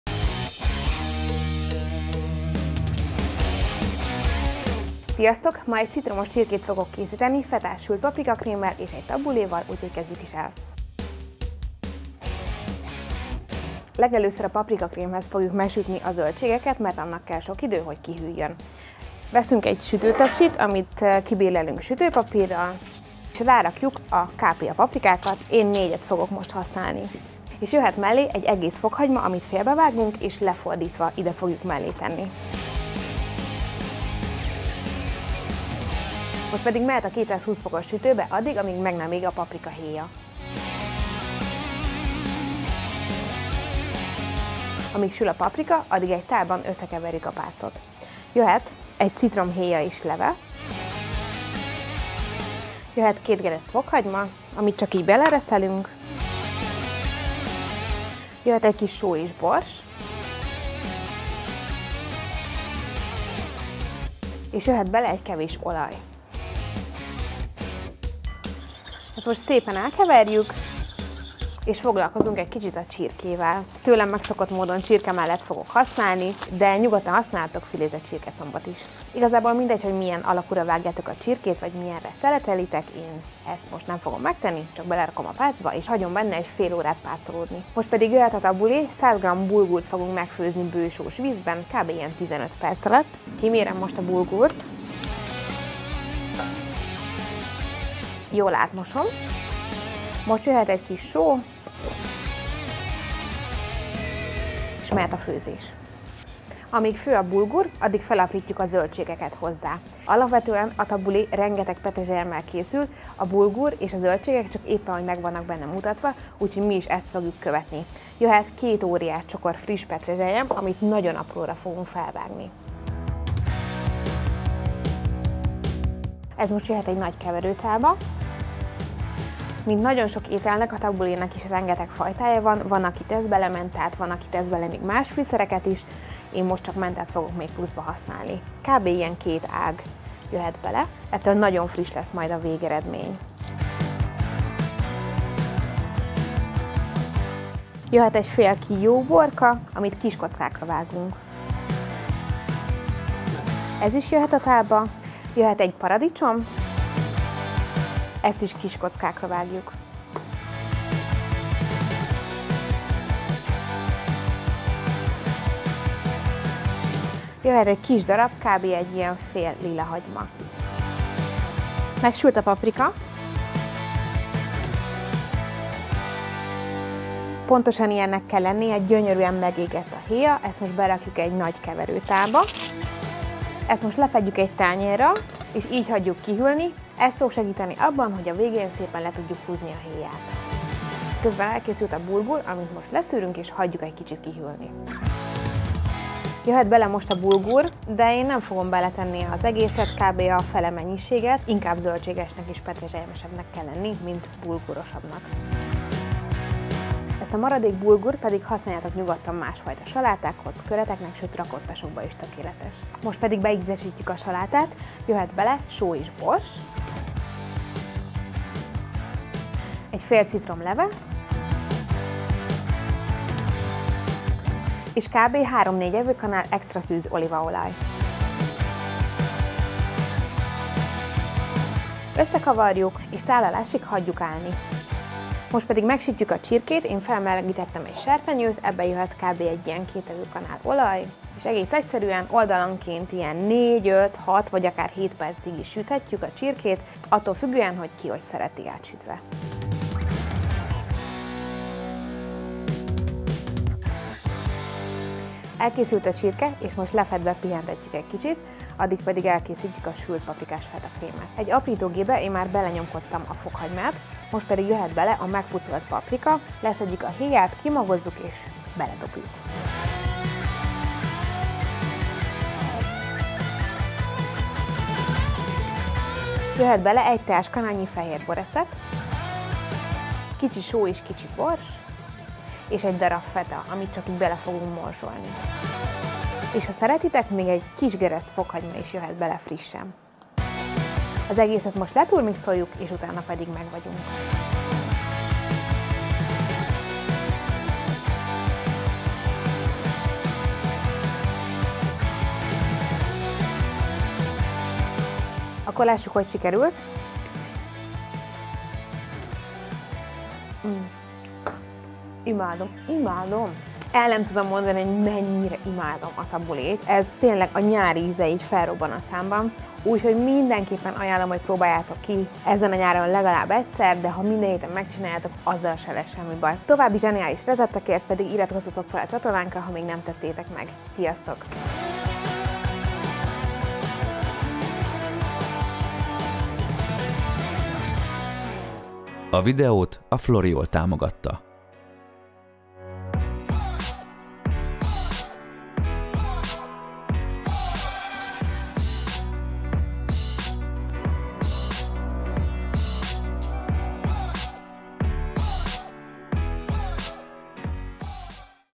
🍋 CITROMOS🍋 csirke tabuléval 🥗
A nyári kaják közt is az egyik legnyáriasabb! 🍋

Intro: (0:00​)
Fetás paprikakrém: (0:15​)
Pác elkészítése: (0:45​)
Csirkemell: (1:15​)
Tabulé: (1:30​)
A paprikák kivétele a sütőből: (2:55)
A tabulé befejezése: (3:20)
A hús megsütése: (3:55)
A sültpaprika-krém befejezése: (4:20)
Tálalás, kóstolás: (4:55)
Outro: (5:43)